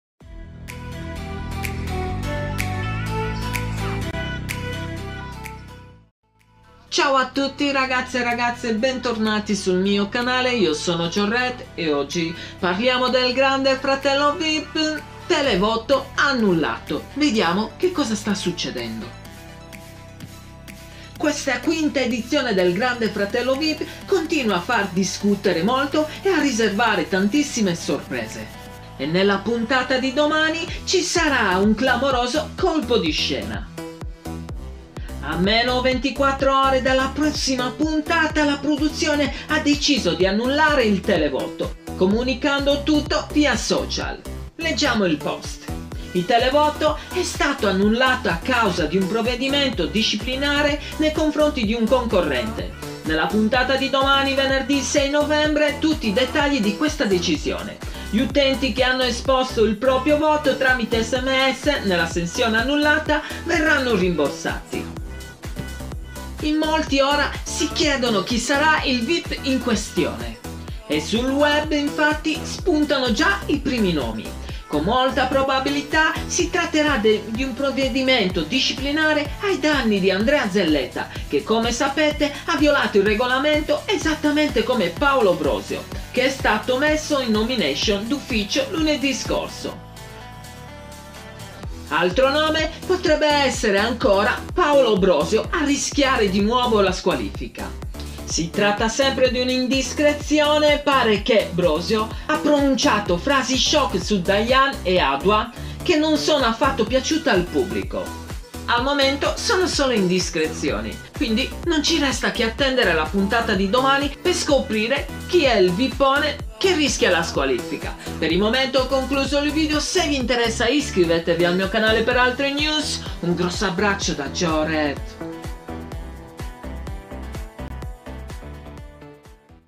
Grande Fratello vip 2020 - televoto annullato - Andrea zelletta e Paolo Brosio squalificati?
Chi sarà il vippone in questione? Sul web ovviamente non sono mancate le prime indiscrezioni e c’è chi ritiene che il concorrente in questione sia Paolo Brosio.

@/04/gf-vip-brosio-adua-dayane/, che non sono affatto piaciute al pubblico. Sarà forse lui il concorrente che subirà i provvedimenti disciplinari?